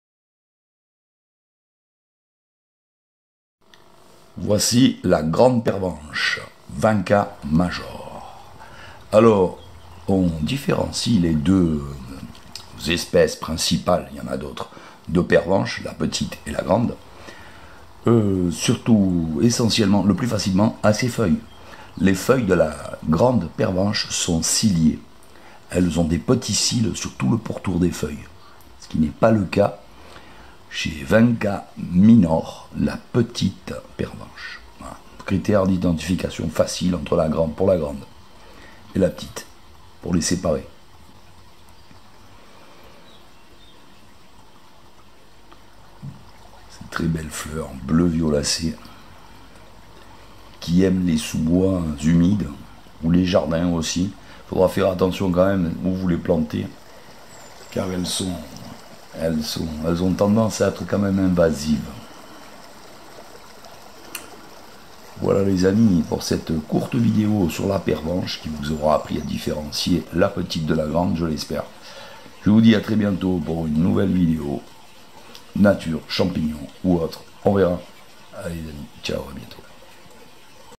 Grande pervenche (Vinca major)
Différencier la Grande pervenche (Vinca major) de la petite pervenche (Vinca minor), deux plantes quasiment identiques à fleurs bleues.